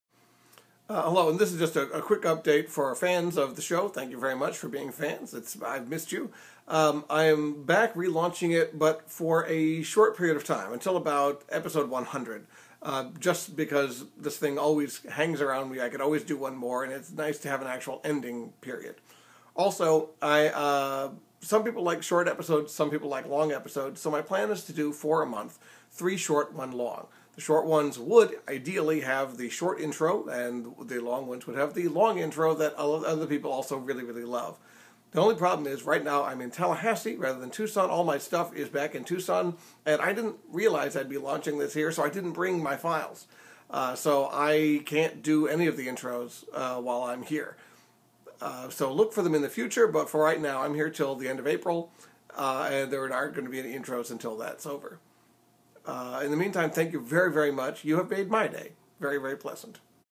Greeting Card Emergency EXTRA: Current Plans for the Show
A short statement where David Ellis Dickerson explains his plans for Greeting Card Emergency...for at least the short term. In case anyone was wondering.